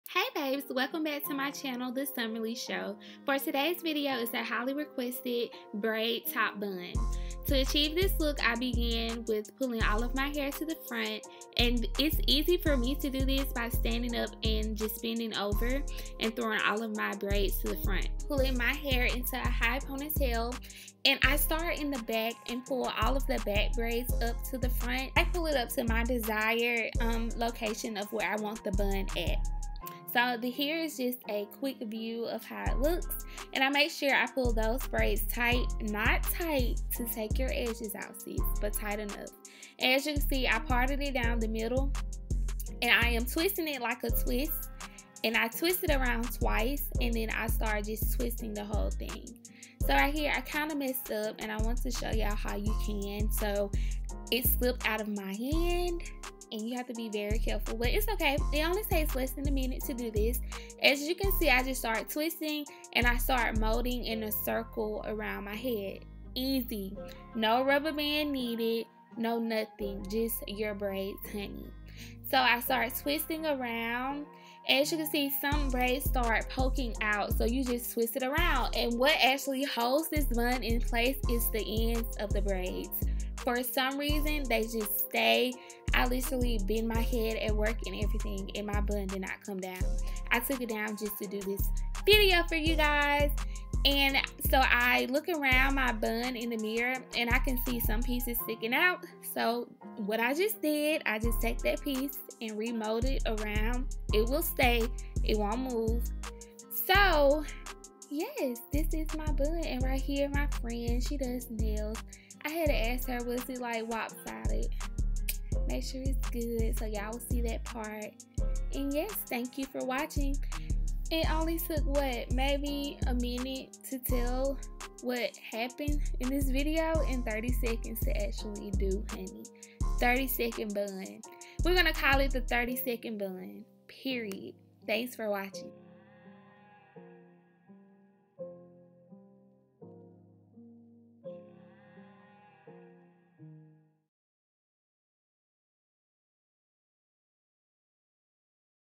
1 MIN KNOTLESS BRAIDS TOP BUN TUTORIAL | ALLTHINGSSOMMERLY
HEY BABBBEEESSSS! I DID THIS VIDEO JUST FOR YOU! Highly requested tutorial on how I achieve my top bun with my knotless braids! I am loving these braids and having so much fun with them!